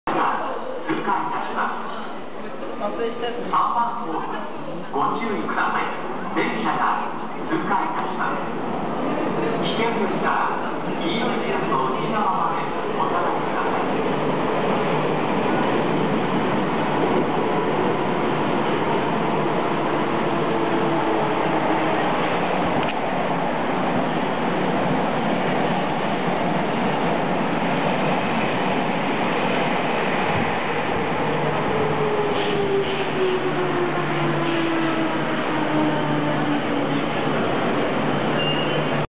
小田急1000形到着&小田急3000形通過
途中で3000形が割り込みます...